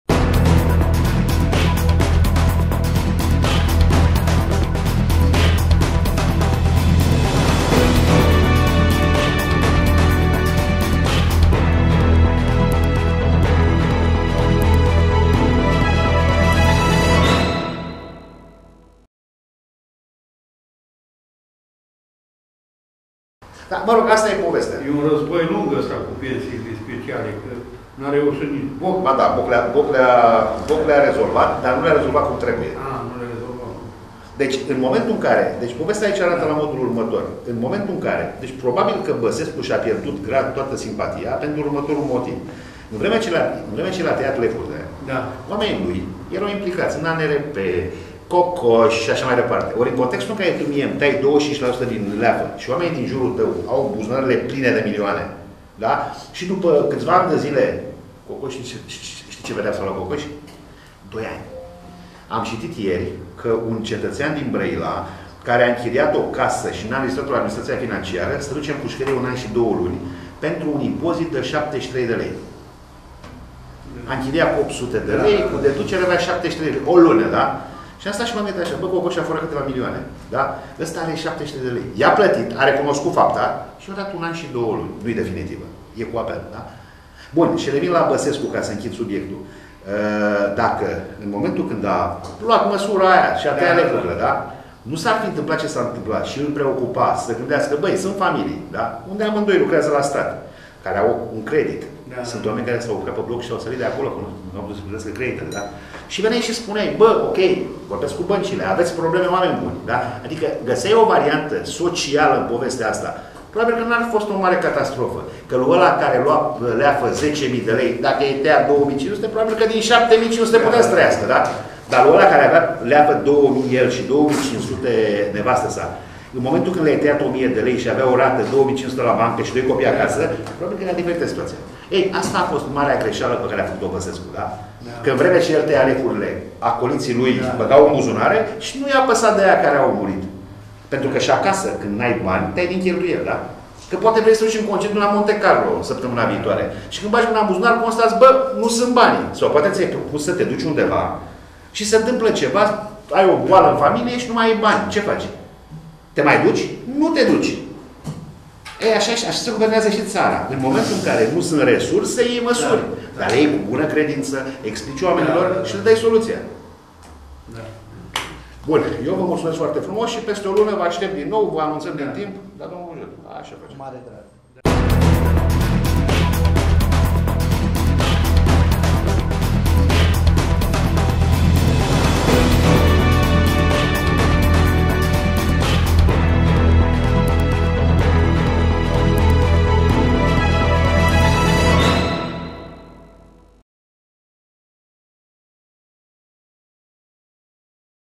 Deputatul Daniel Fenechiu despre ce greseli a facut fostul Presedinte Traian Basescu
Cu stimă,
echipa Exclusiv

Vizionare plăcută !